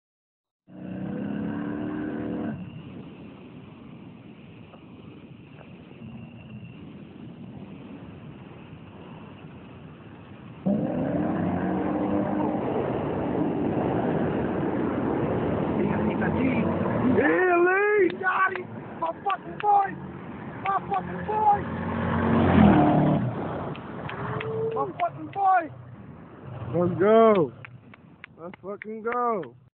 Bowtie Boss lee vs phi inc nard
My first race on my new motor...motor to motor now i won this one we tied now to break the tie